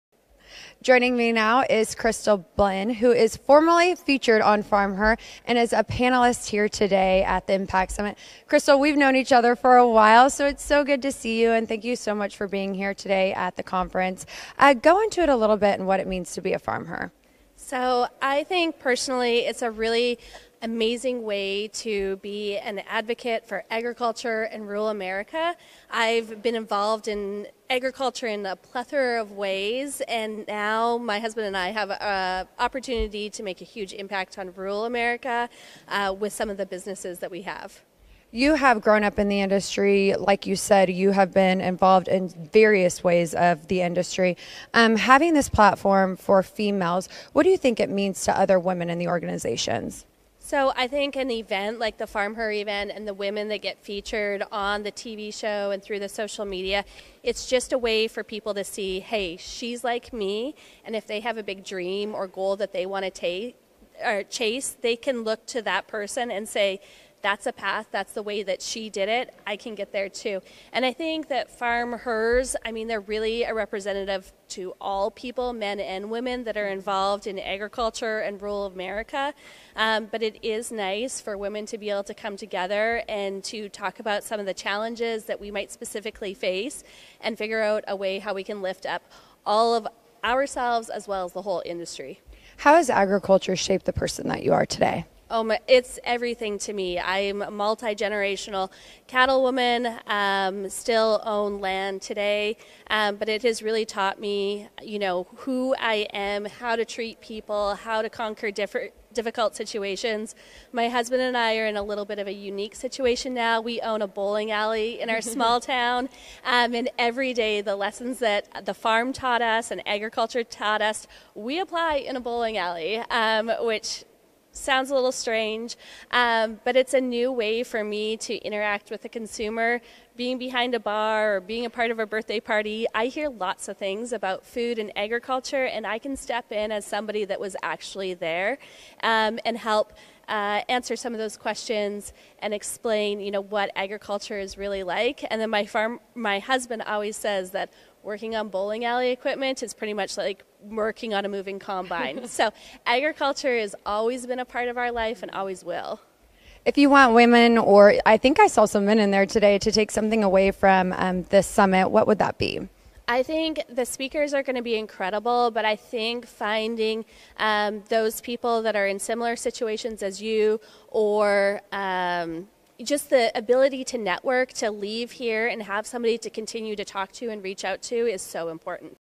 🌟 NEW YEAR, NEW GOALS! 🌟
🌟 Reflect, Grow, and Set Goals 🌟 The FarmHer Impact Summit presented by @nationwide was a day filled with so many inspiring stories like that of FarmHer and cattlewoman @crystal.blin! 👩‍🌾 💚 🐄 In an interview with FarmHer host @kirbeschnoor, Crystal says growing up on a farm helped shape her into the FarmHer she is today! And as she continues in that legacy, Crystal continues to empower other women to pursue their dreams in the industry, too. 💪🔌 Let's continue to support each other, make a positive Impact on Rural America & share our favorite moments from the Impact Summit!!! 🙌🌾